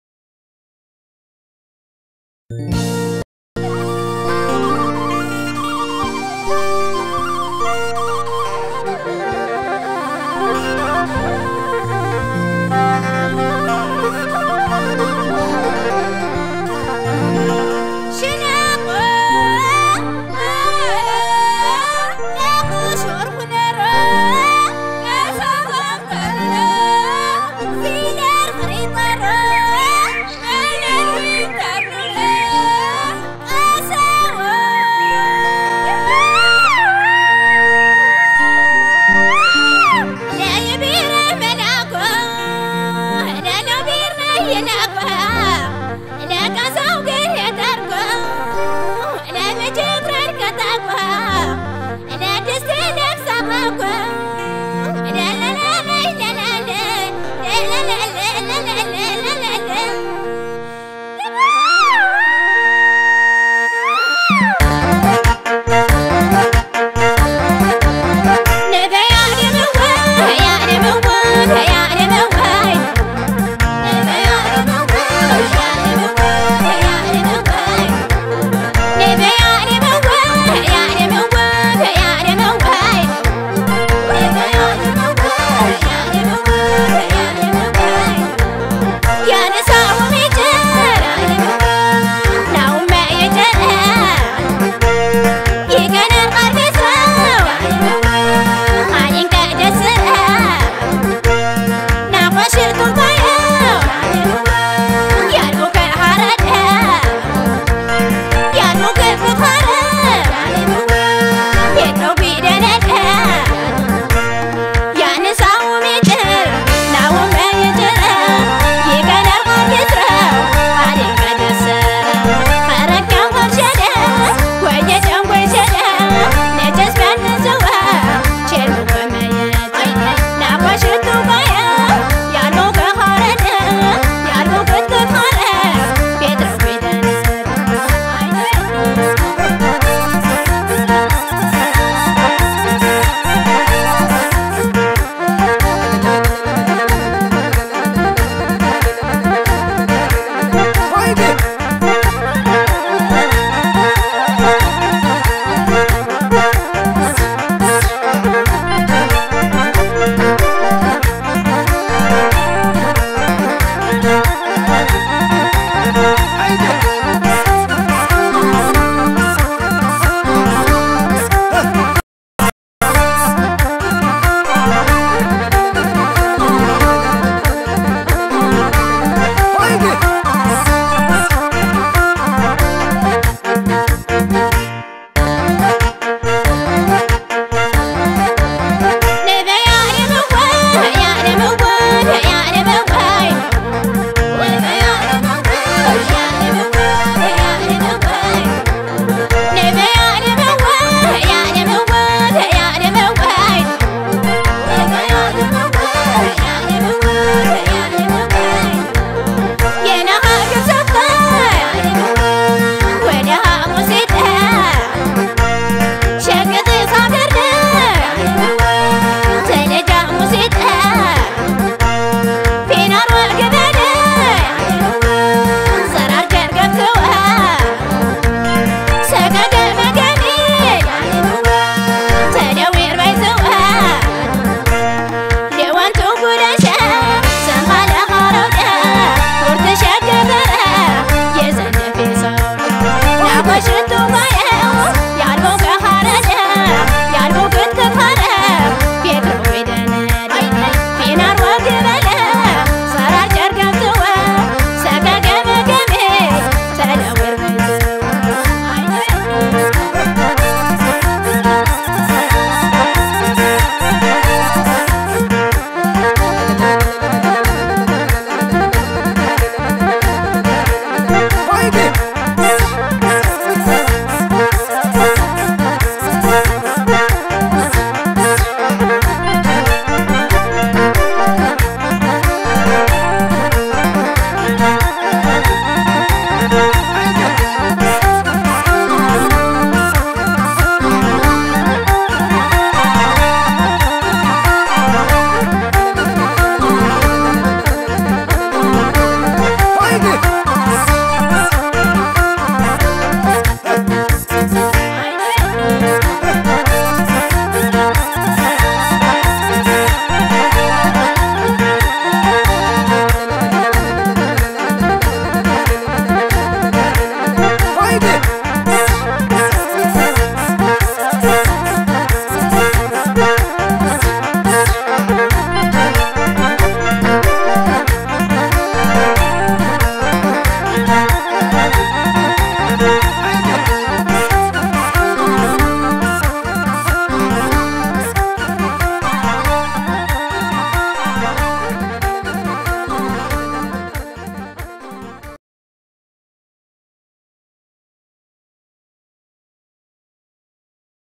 Best Bahilawi Zefen and Eskista from Sekota by Awetu Eyasu and pals
This song too has one of the best Masinko tunes from the singing group in Sekota Ethiopia. The language of the song is Agewugna as it is spoken in the northern part of Wag. Enjoy!